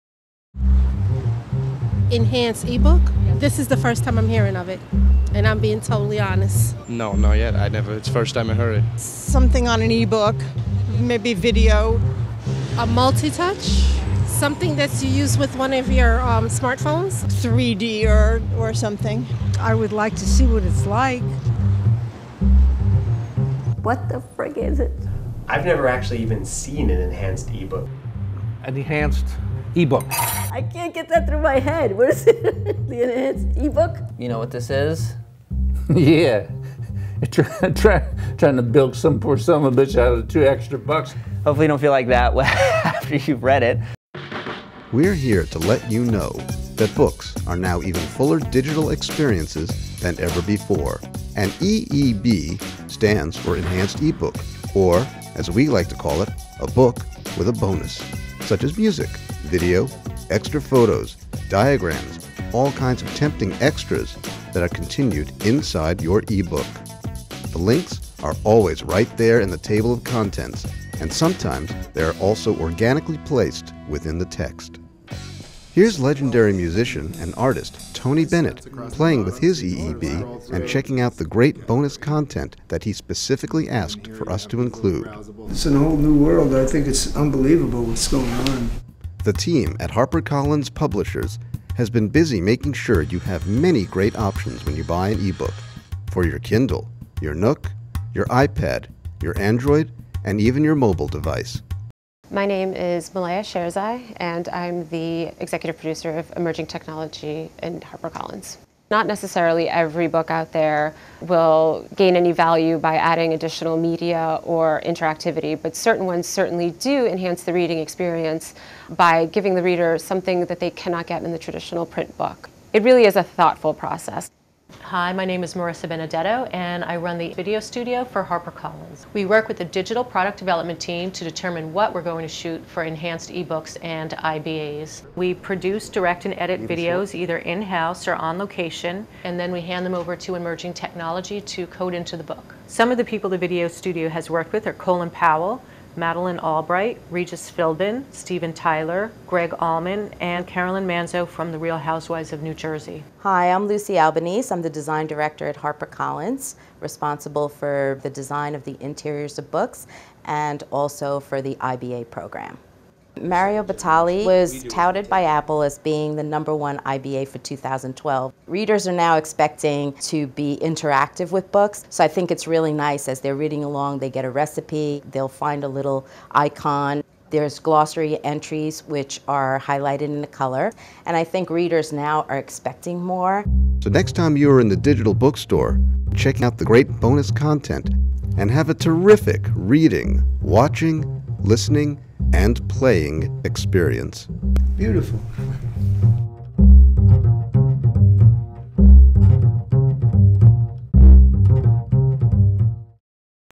What Is an EEB?
What is an EEB? An EEB is an Enhanced E-Book—or, as we like to call it, a book with a bonus! Enhanced e-books include multimedia and bonus content not found in standard e-book editions.